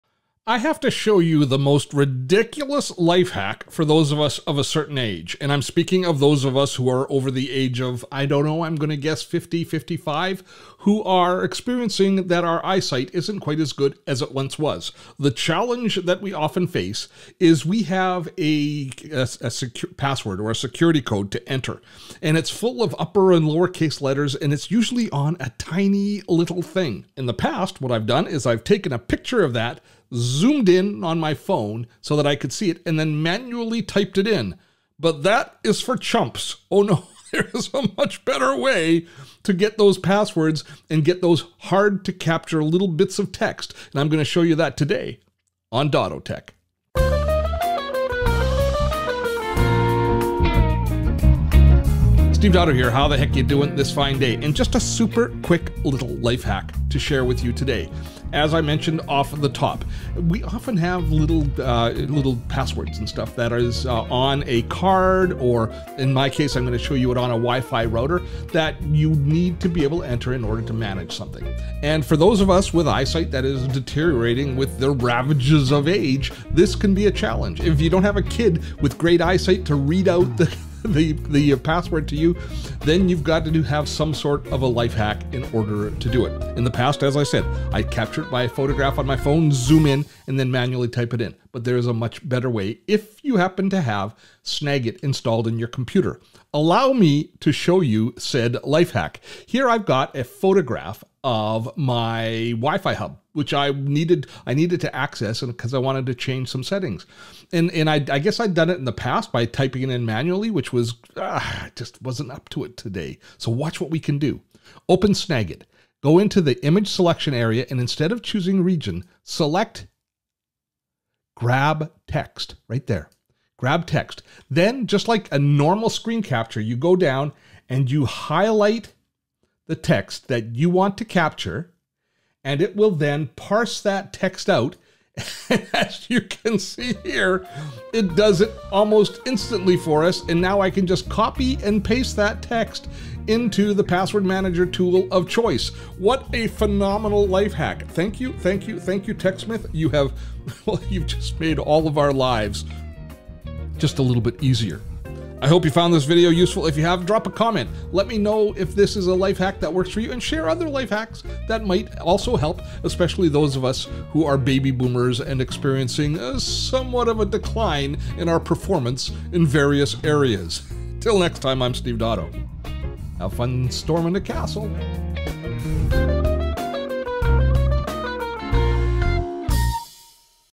Grab Text from Images and Pictures?! You Want to do This!
A simple but effective life hack. Input serial numbers and passwords from the tiniest of sources.
Using Snagit we can extract text from any image, photo or even video.
- Take a picture of text you need to input, such as a serial number or password.
- Grab it using Snagit, and choose "grab text"
- Copy and Paste
Snagit works on both Mac and Windows.

📹  Steve’s Gear:
→ Audio Mixer
→ Green Screen